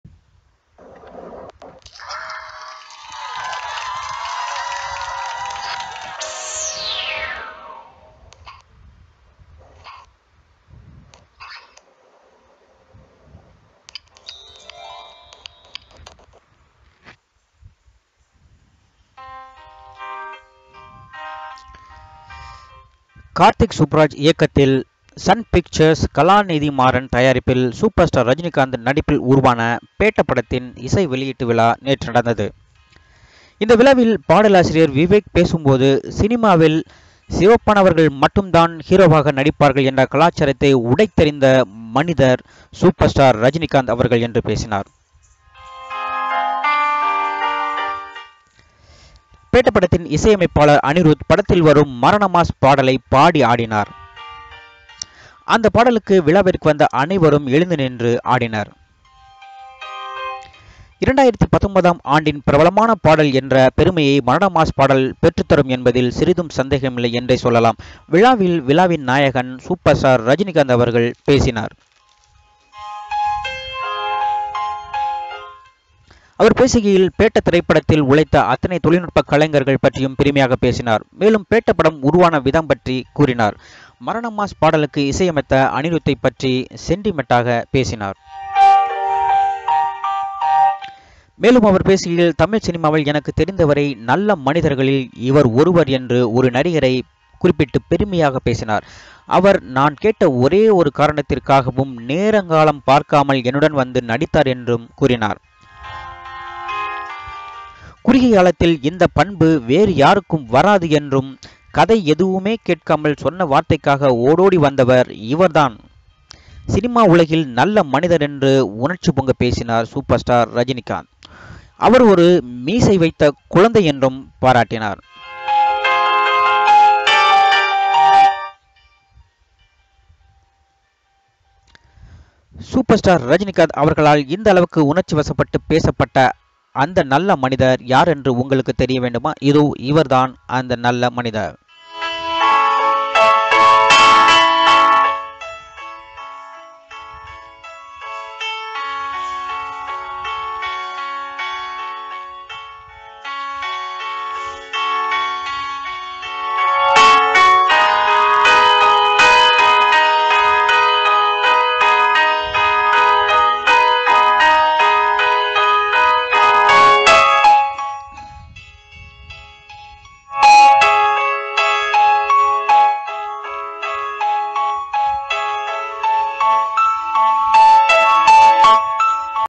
தமிழ் சினிமாவில் இவர் தான் 'நல்ல மனிதர்' - பேட்ட இசைவெளியீட்டில் ரஜினி பெருமிதம்
Please watch this video which is about Super Star Rajinikanth's speech "He is the one of the Good Man in the Tamil Cinema".